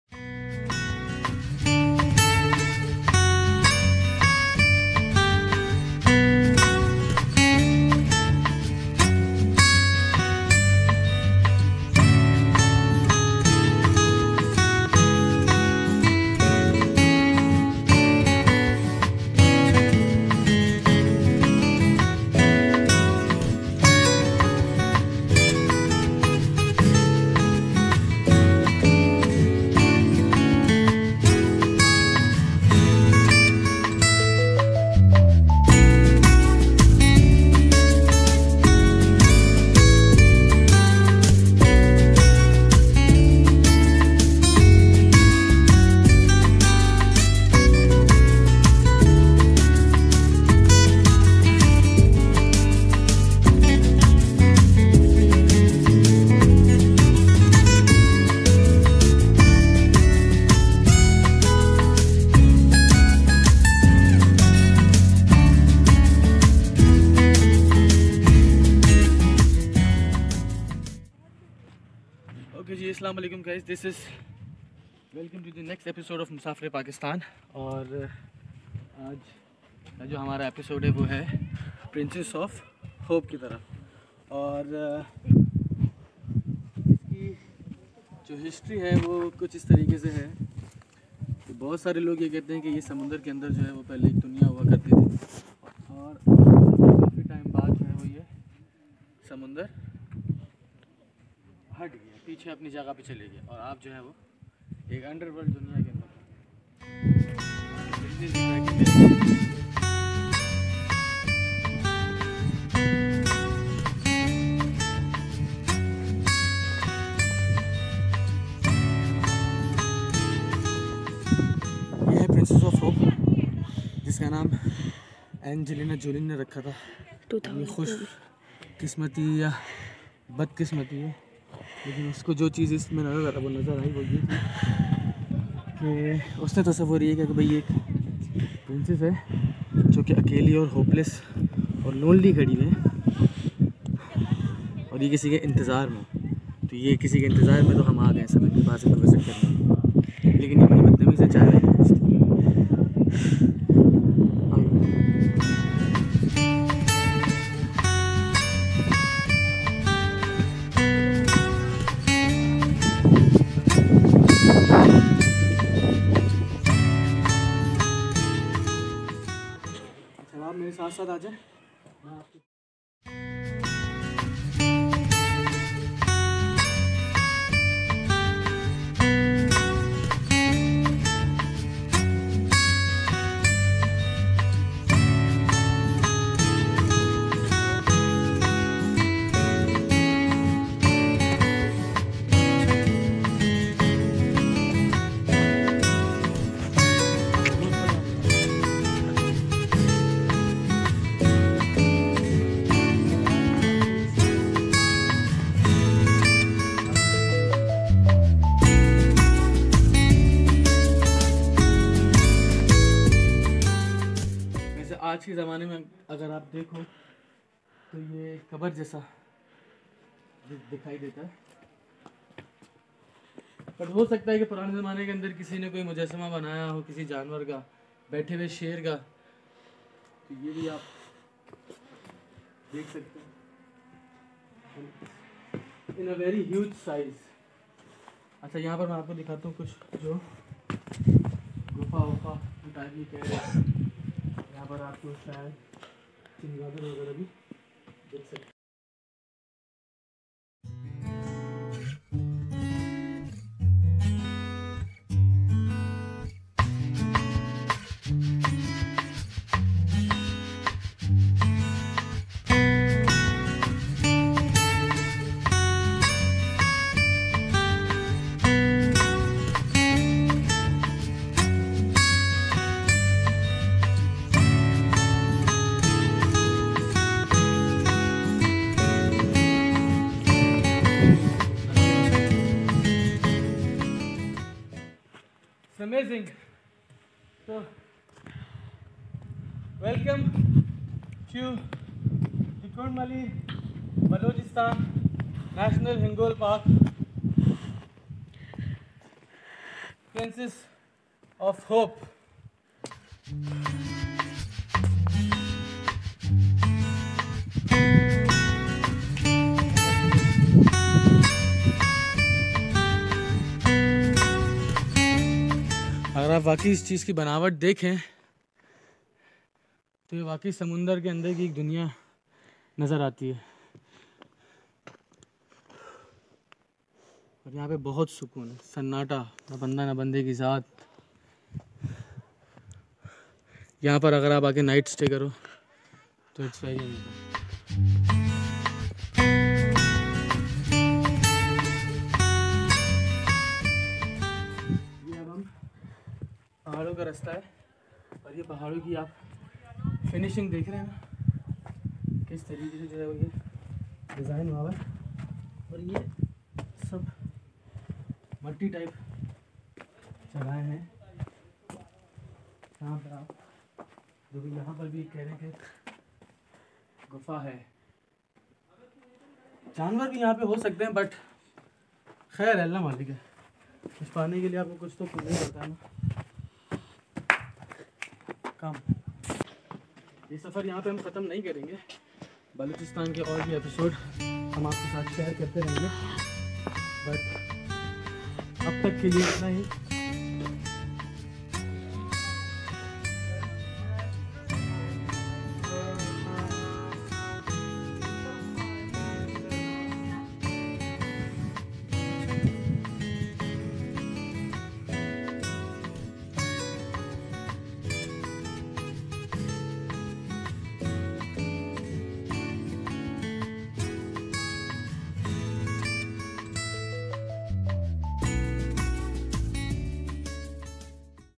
BALOCHISTAN | Pakistan| PRINCESS OF HOPE | Angelina Jolie gave the Name
EPISODE# 04 (PRINCESS OF HOPE)

Small description about this video is that, it is naturally build sculptures in Balochistan before Ormara Beach, You can find out different and different types of sculptures there's , seeing them you will wonder how ad who made these huge and well made sculptures, it is actually give you imagination that there are houses, places, temples, graves, statues made By Nature..

We should be be ThankFull to Almighty ALLAH for Everything.

I HOPE YOU ENJOY MY SMALL TRIP TO PRINCESS OF HOPE.

VLOGGER# IBAD MANGALWAL
VIDEOGRAPHER# MRS. IBAD